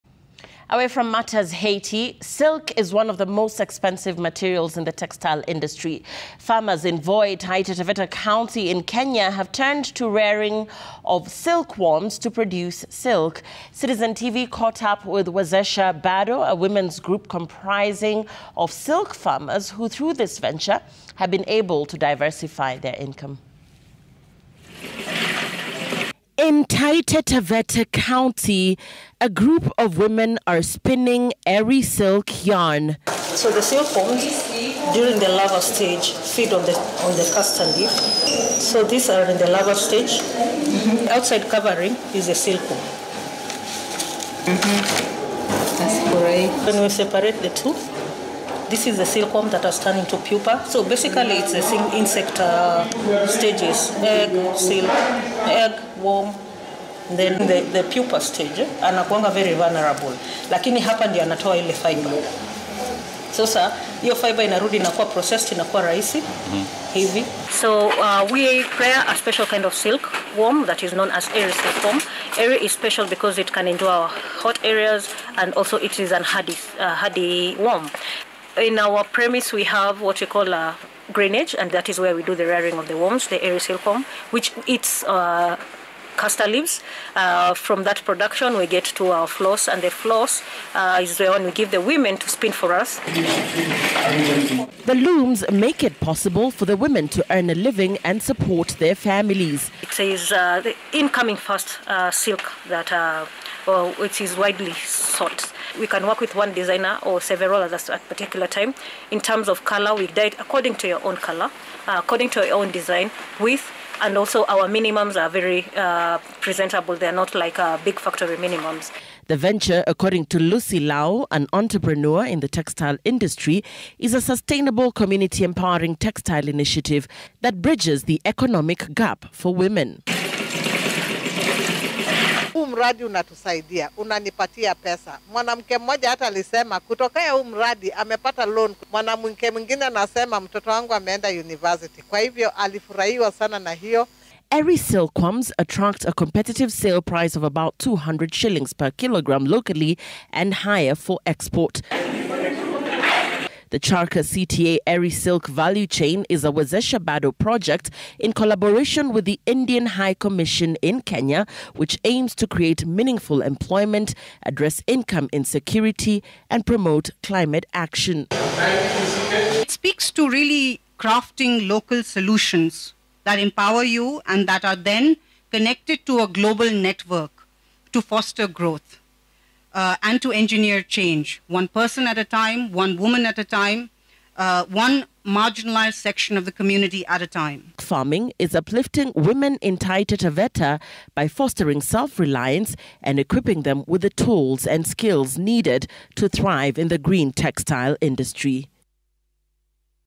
Silkworms farming in Voi to produce silk
They can be reared in simple structures and equipments